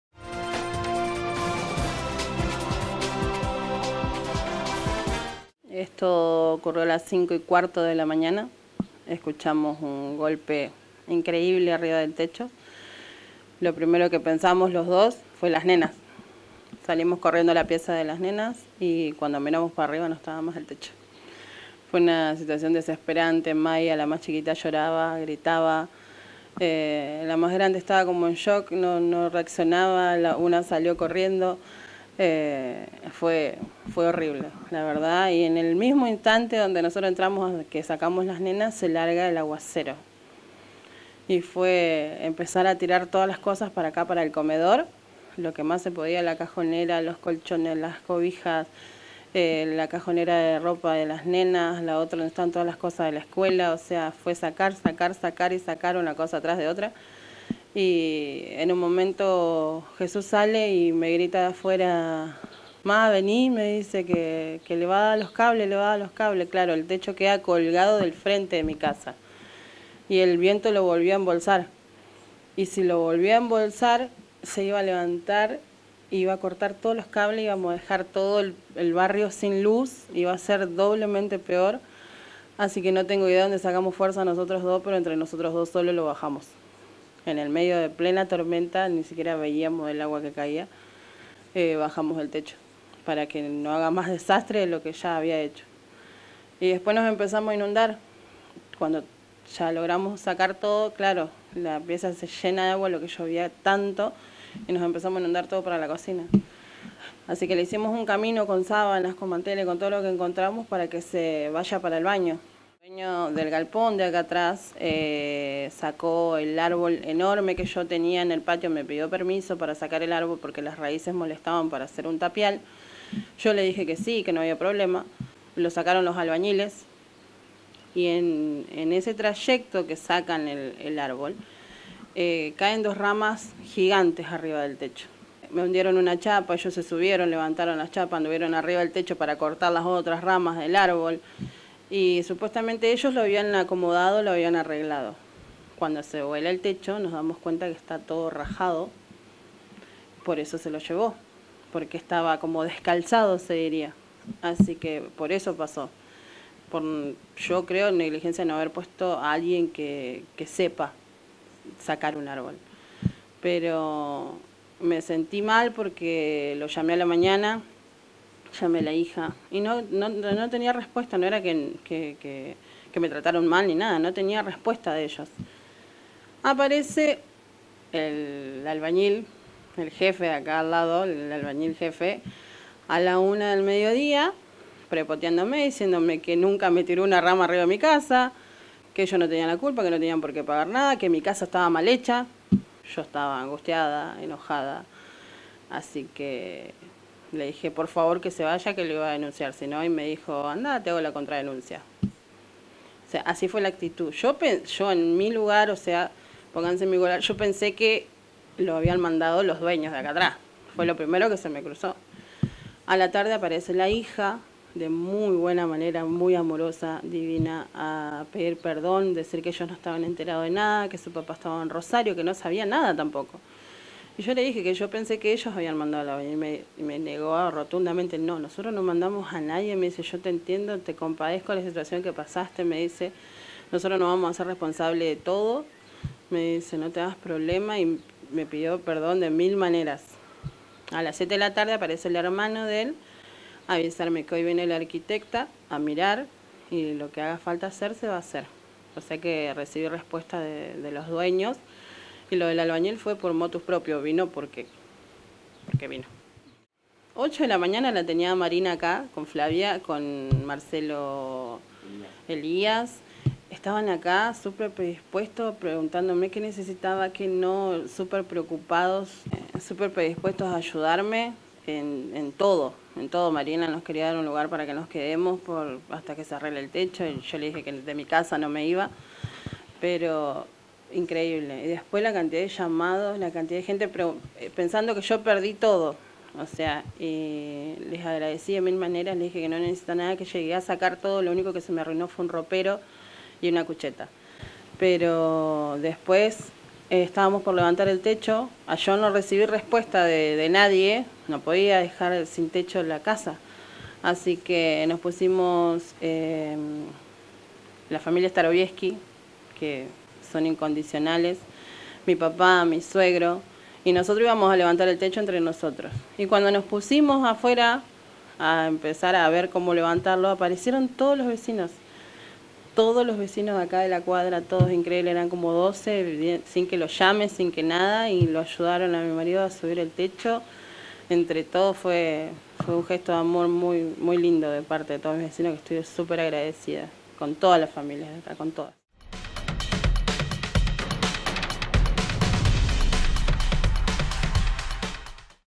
NOTA JESICA SANTA CRUZ - LA TORMENTA LE VOLÓ PARTE DEL TECHO DE SU CASA
VECINA DE CARLOS PELLEGRNI